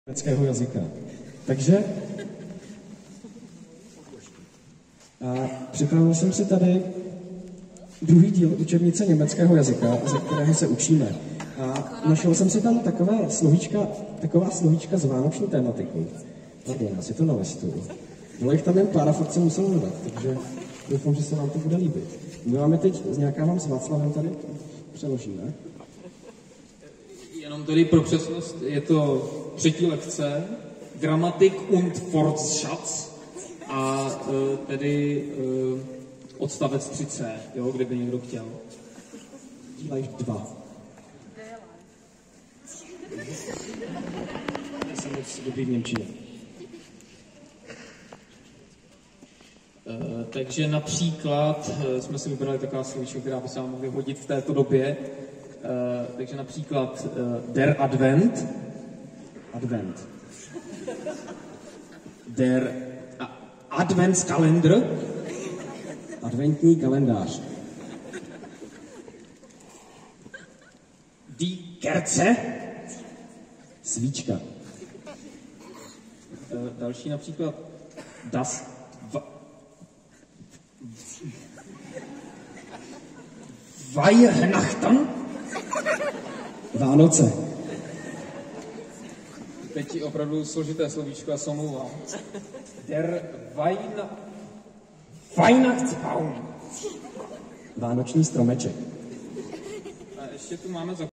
Slovníček německých adventních pojmů
Ve čtvrtek 15. prosince 2022 se v chrámu sv. Mikuláše v Českých Budějovicích konal tradiční adventní koncert smíšeného pěveckého sboru Mendík, který působí při Gymnáziu Jana Valeriána Jirsíka pod vedením sbormistryně Mgr. Olgy Thámové.